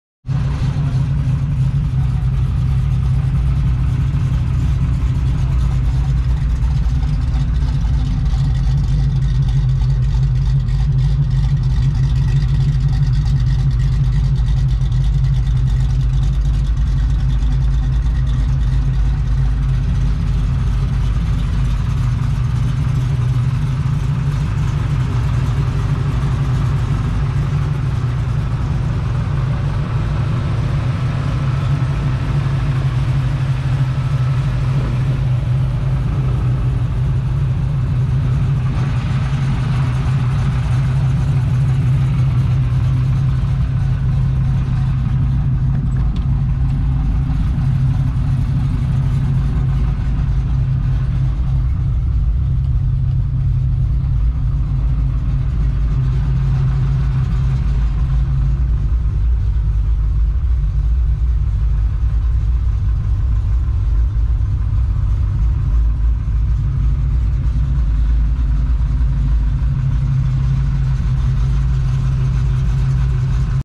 1972 Chevrolet Nova Walk-around Video
The Chevrolet Nova, or Chevy II, is a compact automobile from the
Bowtie's lineup, and it ran between 1962 and 1979, returning for three
more years between 1985 and 1988 before bowing out for good. The
third-generation Nova, which saw life between 1968 and 1974, is
arguably the most prolific when the Nova would seriously contend with
prolific names in the muscle car segment. This 1972 Chevrolet Nova is
an excellent third-gen example, as it's a fully restored example with
a newer and more potent heart. With just 100 miles since its second
life, it awaits the right owner. Will that be you?